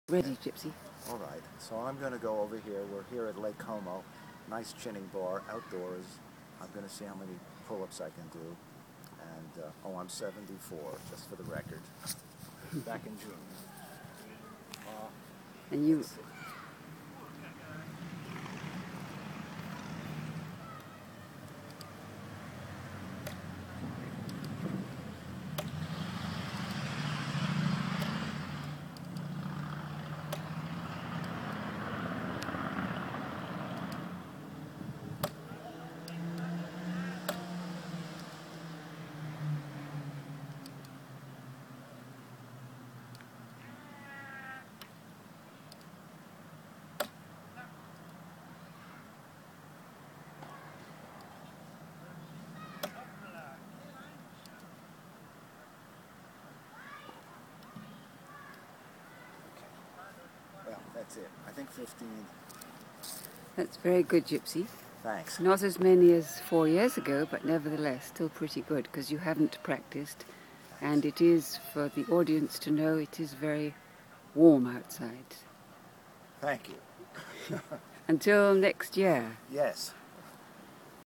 Age 74 Chins 15
Just returned from Lake Como, Italy. There was a nice chinning bar outside the hotel and it was a hot day in September. The Wife took a video of my doing my chins.